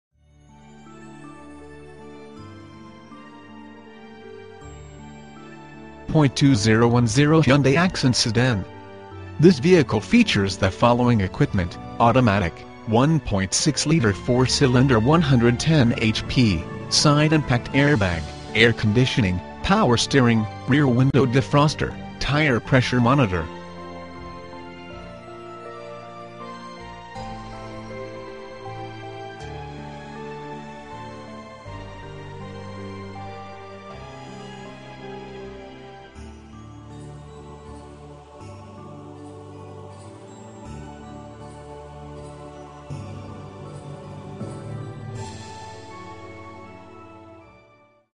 2010 Hyundai Accent - Cals Park -n- Sell
900 E. Dowling Rd

For Sale: 2010 Hyundai Accent
VIN: KMHCN4AC8AU421565
Engine: 1.6-Liter 4-Cylinder 110HP
Drivetrain:
Transmission: automatic
Mileage: 33,404
Color: WHITE (exterior)  (interior)

Features:
2010 Hyundai Accent Sedan. This vehicle features the following equipment: automatic, 1.6-Liter 4-Cylinder 110HP, Side Impact Airbag, Air Conditioning, Power Steering, Rear Window Defroster, Tire Pressure Monitor